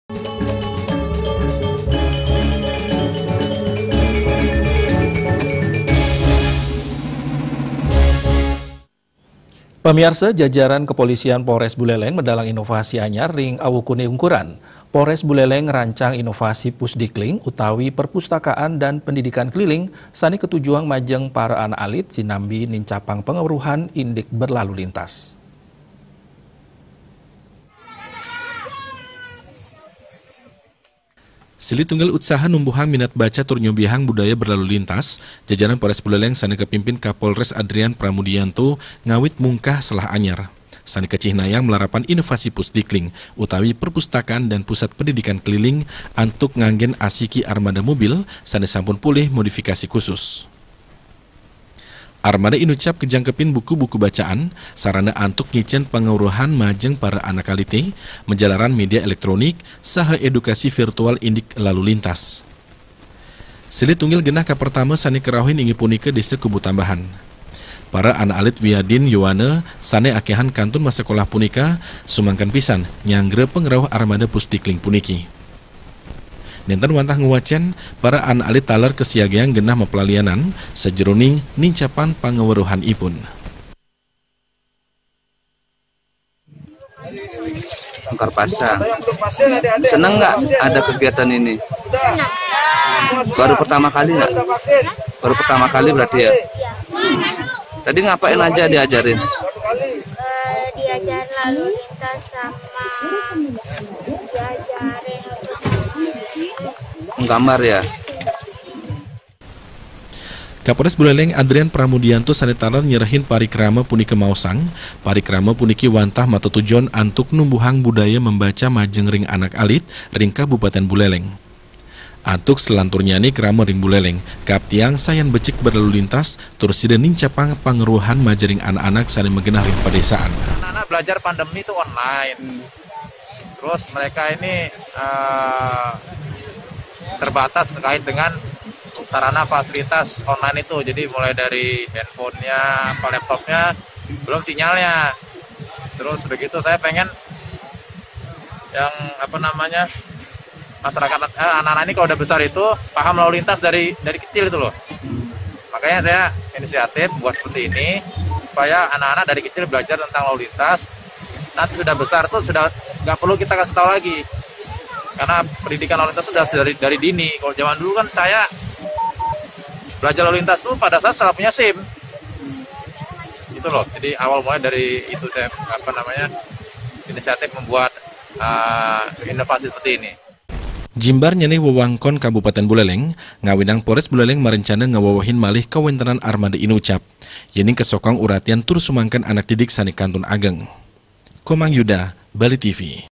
INOVASI PUSDIKLING OLIH POLRES BULELENG
JAJARAN KEPOLISIAN POLRES BULELENG MEDALANG INOVASI ANYAR RING AWUKUNE UNGKURAN. POLRES BULELENG NGARANCANG INOVASI PUSDIKLING ,UTAWI PERPUSTAKAAN DAN PENDIDIKAN KELILING , SANE KATUJUANG MAJENG PARA ANAK ALIT, SINAMBI NINCAPANG  PANGAWERUHAN INDIK BERLALU LINTAS.